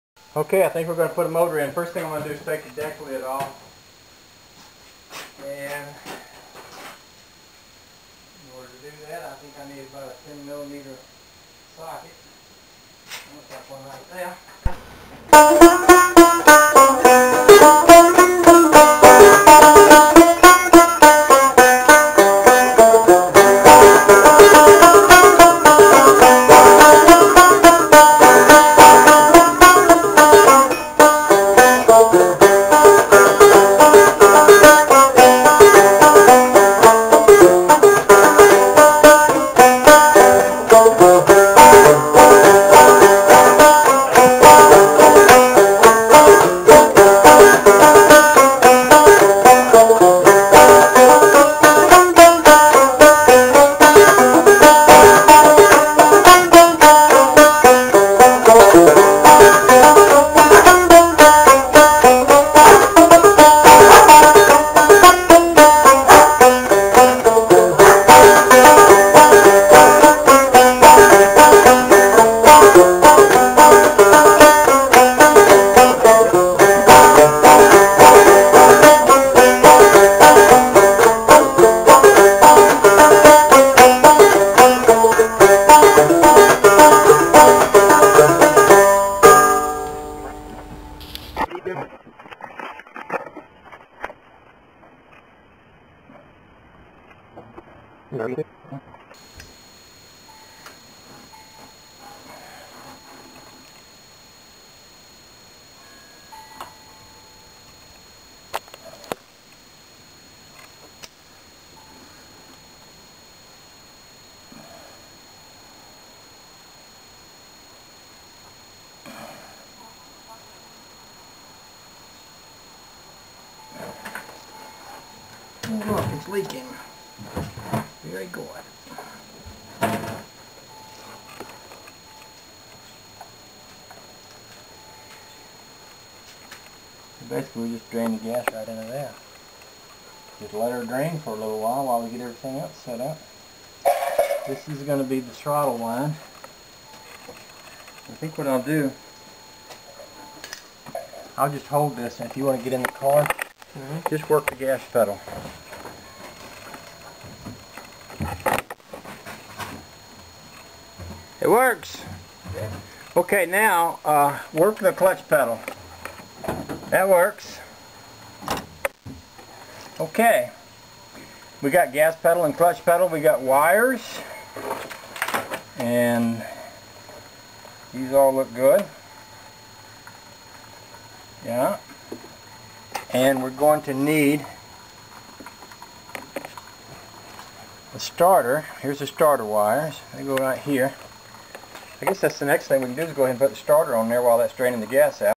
1964 VW hood off first engine install video
I taped 7 videos while my son was visiting, of us putting the motor in the VW bug.  The visual quality isn't that good I'm afraid and I should have put them up before I shot videos of the motor running in the car, but it was a fun time working with him and Trish in the garage, so I thought I'd go ahead and show them now that I have had time to pull them up and load 'em.  Some might find these too long and boring.  Feel free not to watch..  Josh said people are interested in the boring details of this stuff, and maybe they are, so for better or worse, here they are, whether you like it or not..  Thanks.